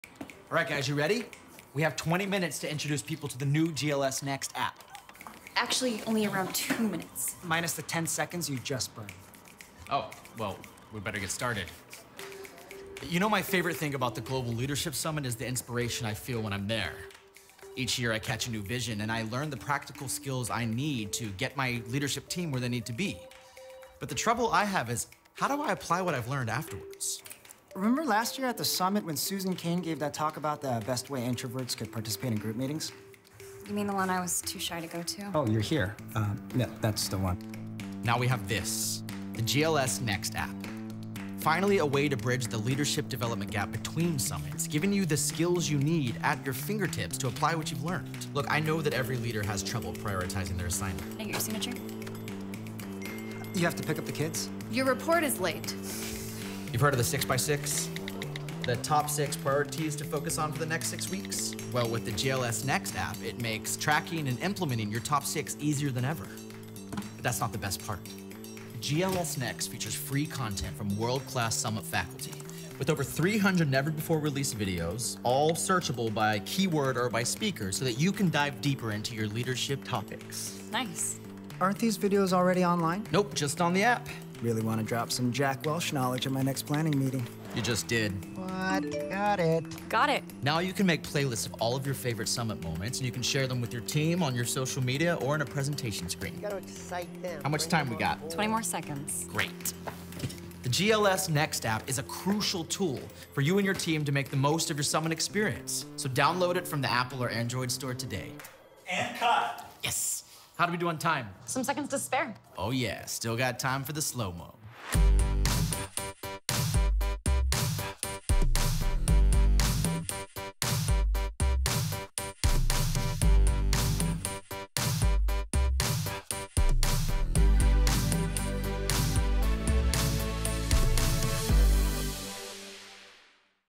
Introducing the GLSnext App
Begin sharpening your leadership skills and unleash the full potential of your team.

The GLSnext app helps keep you engaged between global Leadership Summits with talks from a world-class faculty.

Summit content and value add resources are delivered throughtout the year through short video segments that are searchable by speaker and topic.

The entire leadership library is at your fingertips and the GLSnext app updates every week with new talks from the GLS faculty from around the world.